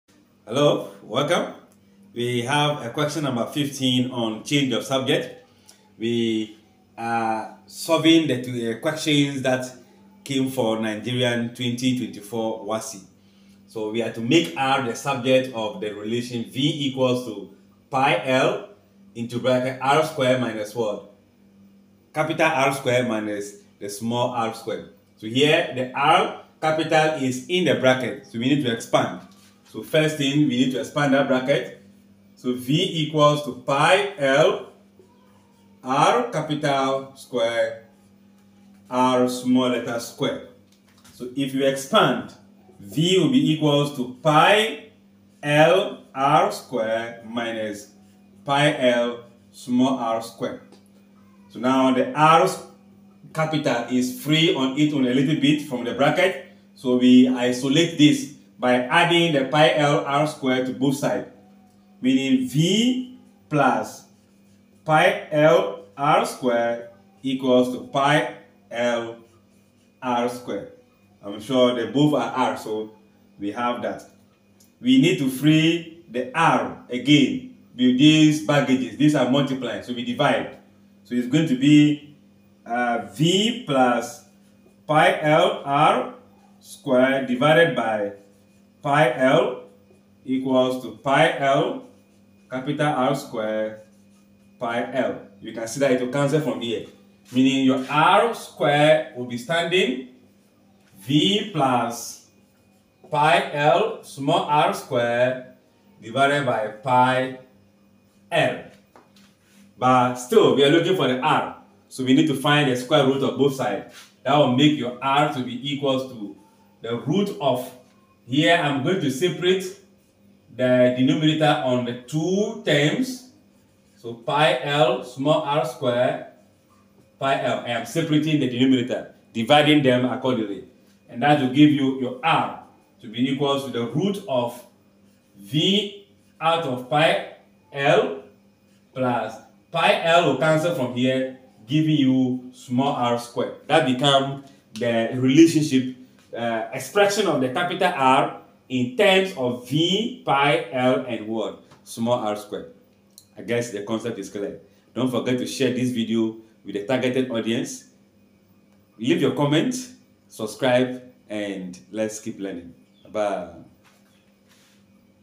2024 WASSCE MAY/JUNE | Change of Subject |OBJ No. 15
Solving question on change of subject